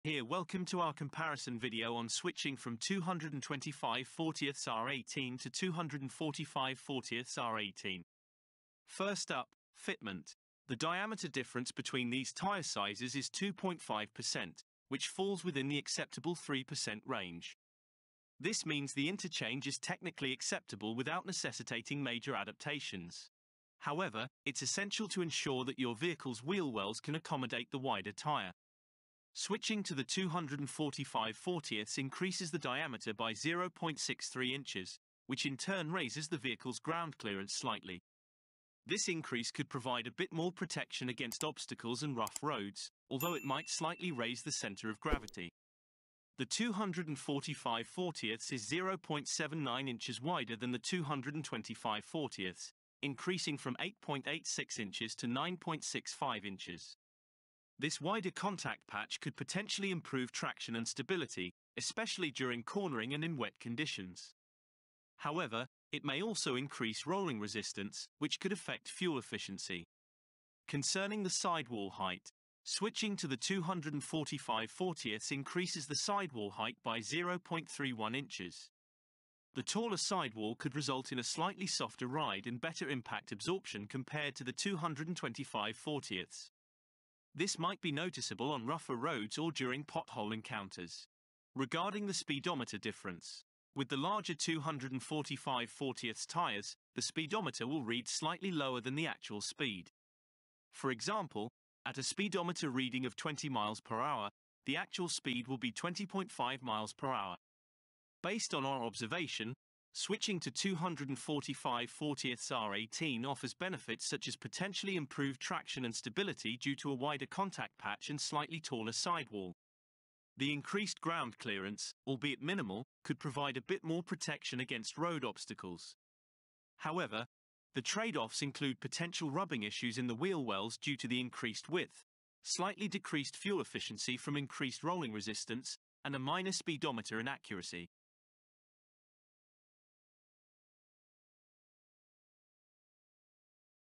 Tire Size 225/40r18 vs 245/40r18
Exploring the differences between 225/40r18 vs 245/40r18 tires? This video dives deep into fitment, height, fuel economy, and handling characteristics.

Our expert analysis helps you understand the pros and cons of each size. View side-by-side comparisons and real-world examples to inform your tire choice.